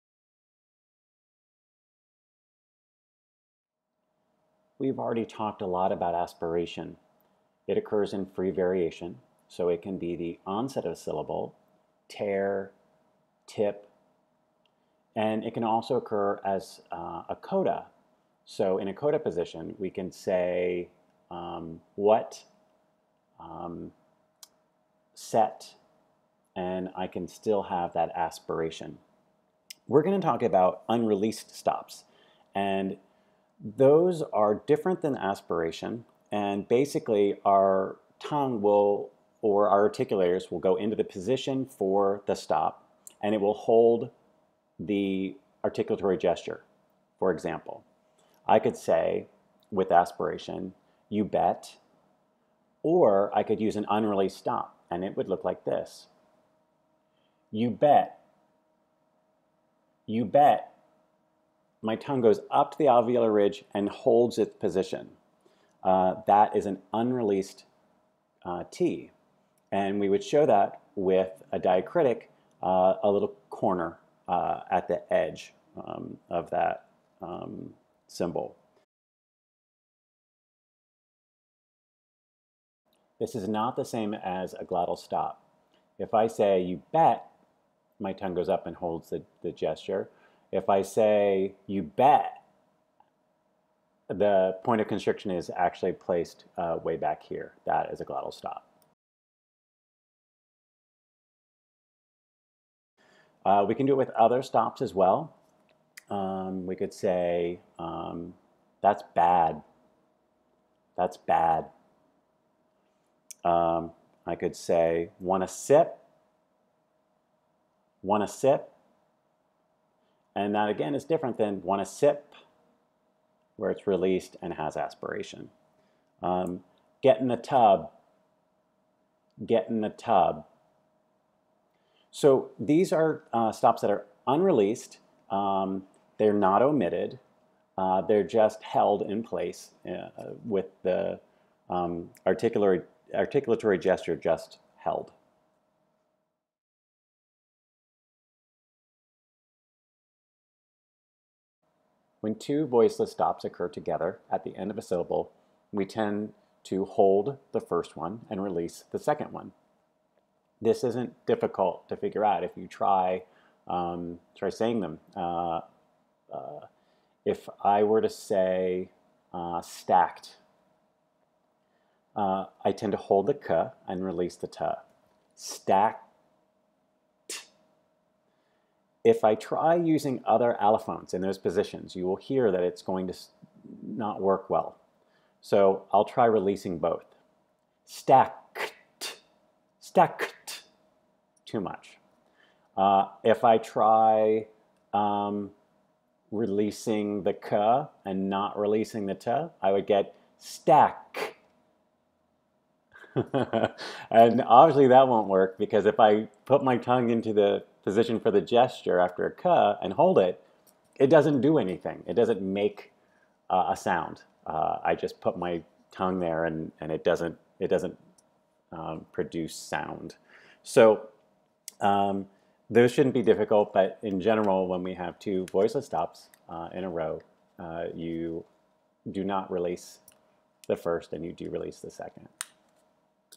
Unreleased Stops
Sometimes the articulatory gesture for stop consonants is held in place (not released). This can be encoded in transcription with a diacritic called a 'corner'.